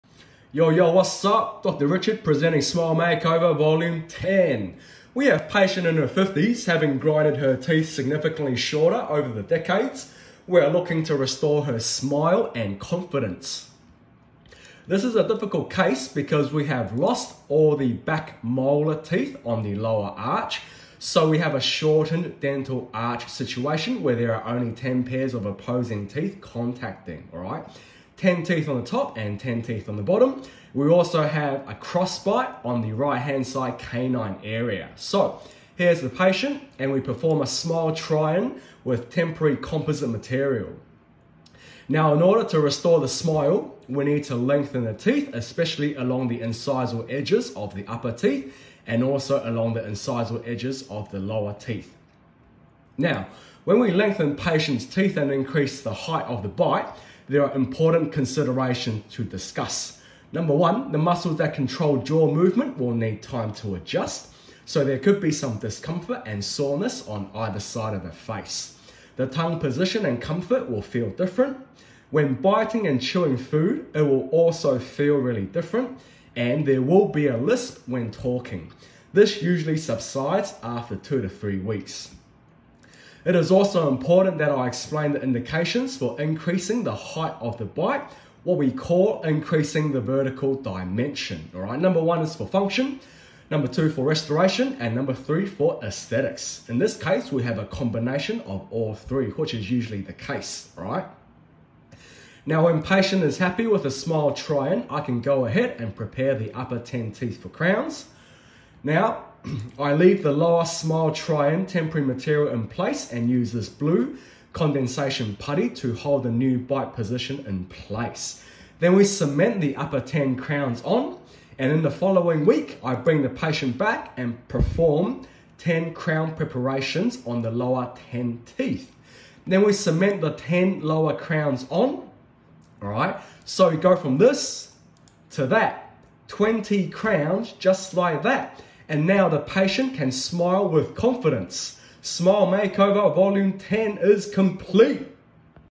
Smile Makeover Australia vol10 with DrRichard Increasing Vertical Dimension of Occlusion Pros Dental
Smile Makeover Australia vol. 10 with Dr Richard

** Title is just 'click-bait' - every title is 'click-bait' - don't take it too seriously 😉

Increasing Vertical Dimension of Occlusion

Video presentation has been simplified to allow for easier understanding. The real case was more difficult and completed after numerous hours of treatment & clinic time.

The 'Height of the Bite' / 'Vertical Dimension of Occlusion' was carefully increased. The new bite position was established using composite material as part of the smile design smile try-in process. This was performed prior to preparing the teeth for the crowns & bridge.

Condensation putty index used to assess incisal reductions. Desensitizing agent applied after teeth preparation to minimize likelihood of teeth sensitivity.

All workflow is digital, utilizing the iTero digital scanner. All Patients leave clinic with a 'temporary'-composite smile after teeth preparation.